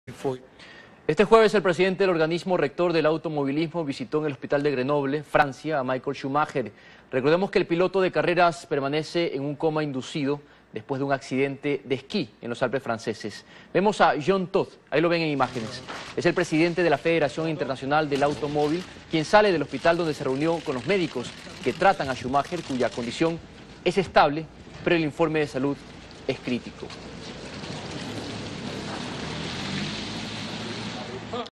Schumacher's condition stable but critical
Michael Schumacher remains in a critical condition in a Grenoble hospital, FIA President Jean Todt said after meeting his doctors. teleSUR